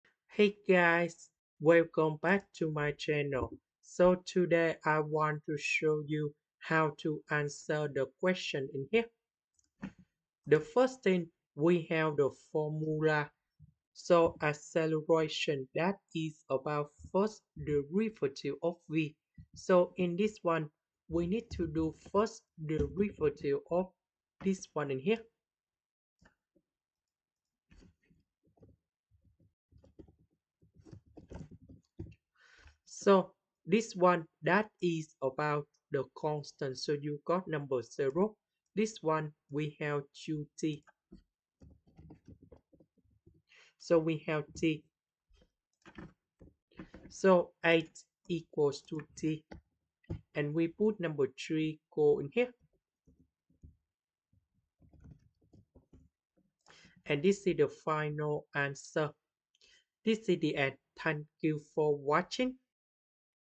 Physics Help: Suppose the velocity (v) of a car at any time (t) is given by the equation v=60m/s+
Full question:
Suppose the velocity (v)  of a car at any time (t)  is given by the equation
v=60m/s+ (0.5m/s^3) t^2.Find the instantaneous acceleration at t=3s